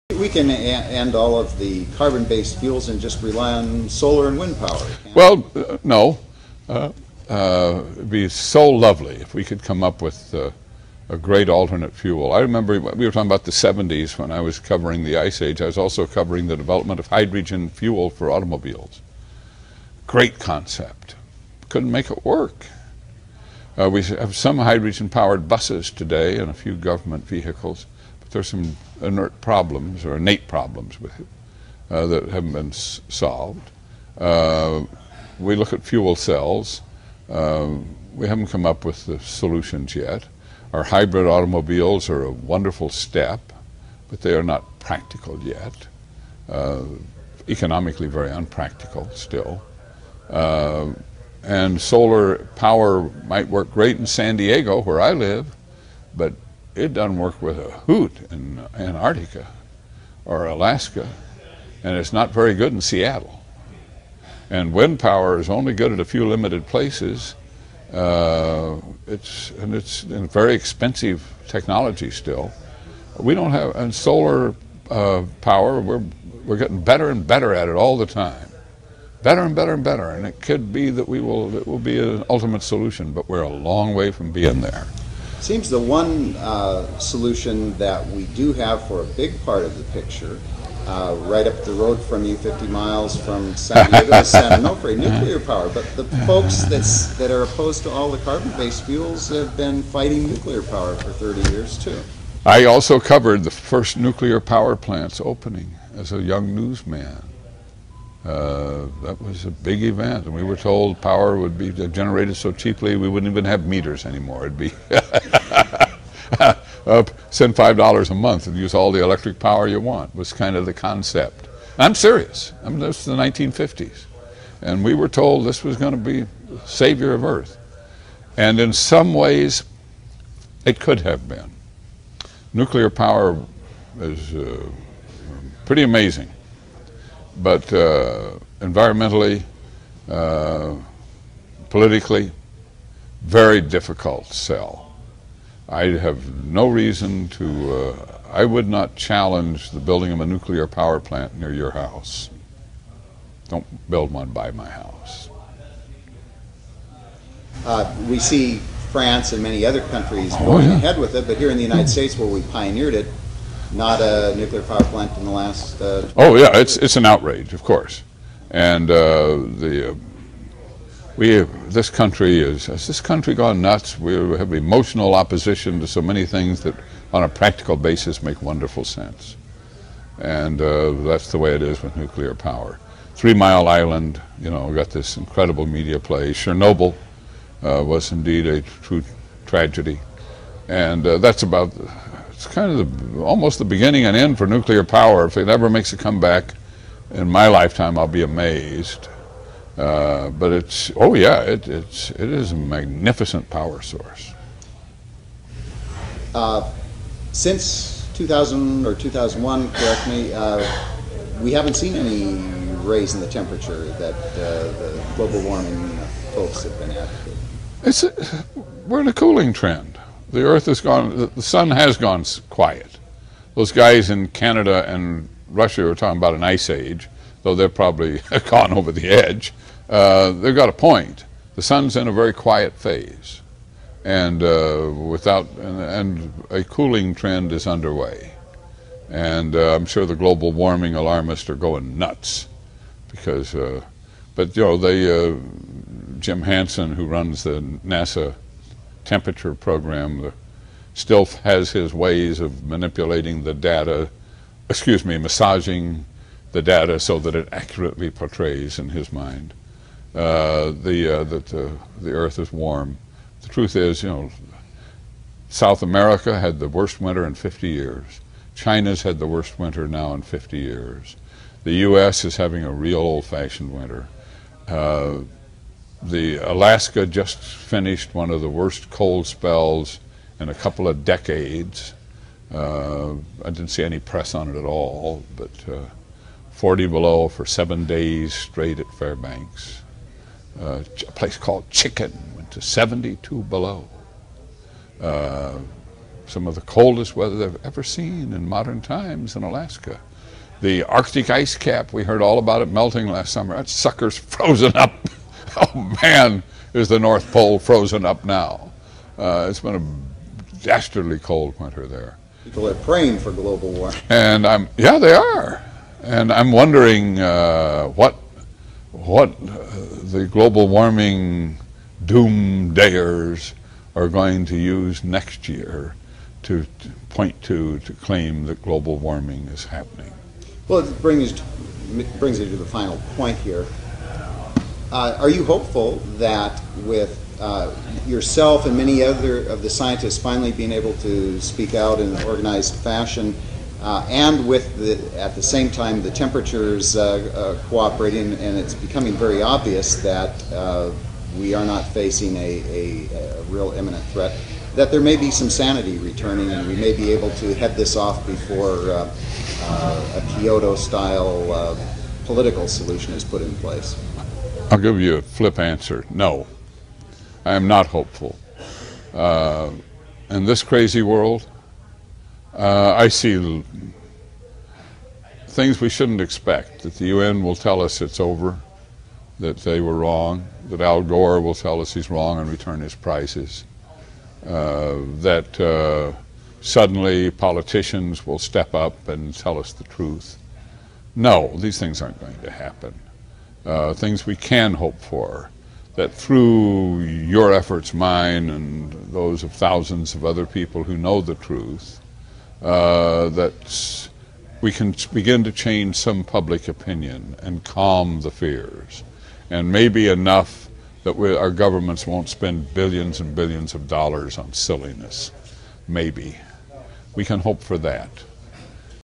Founder of The Weather Channel Slams Global Warming! (Part 4)
Co-founder of The Weather Channel, John Coleman speaks out against global warming.